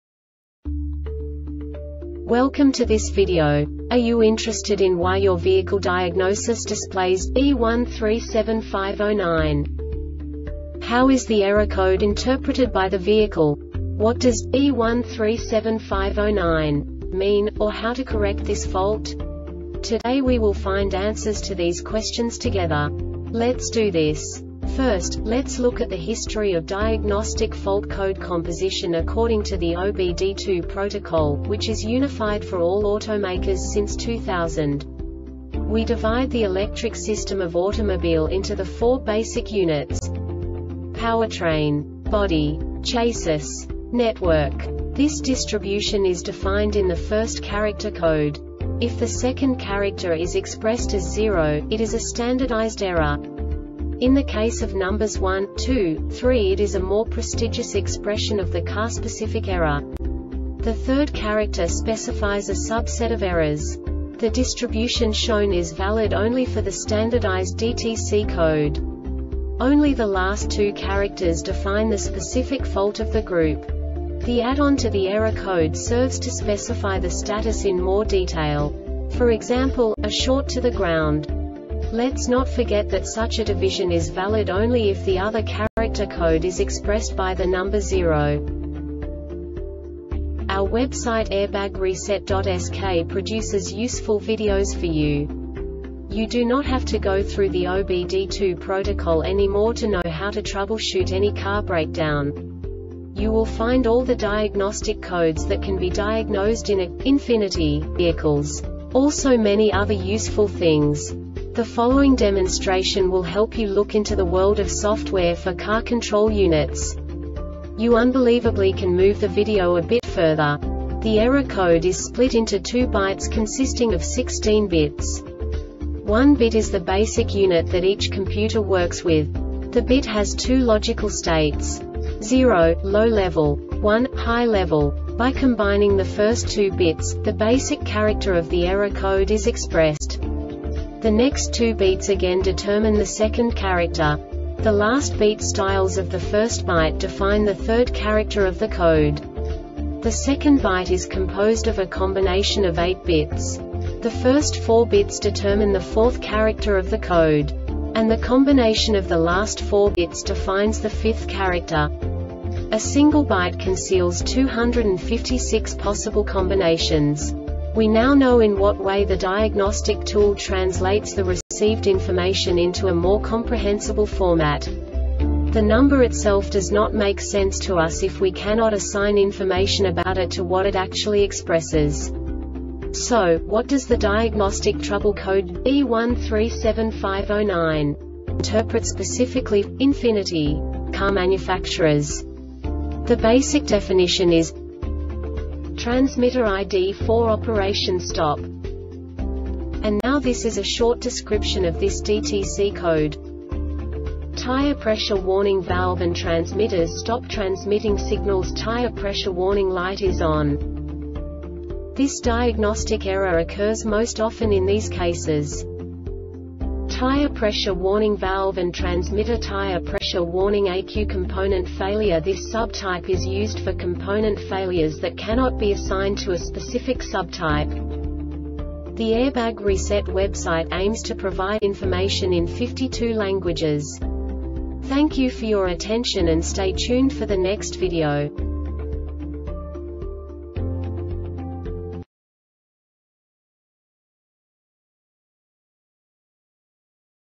DTC Ford B1375-09 Short Explanation
Contents:
0:21 Basic DTC analysis according to OBD2 protocol standard.
1:48 Insight into programming
2:58 A diagnostically understandable description of the Diagnostic Trouble Code
3:05 Basic error definition

Make:
Ford
Code:
B1375 09
Definition:
Transmitter ID4 Operation Stop
Description:
Tire pressure warning valve and transmitters stop transmitting signals.Tire pressure warning light is ON.
Cause:
 Tire pressure warning valve and transmitter Tire pressure warning ECU
Failure Type:
Component Failure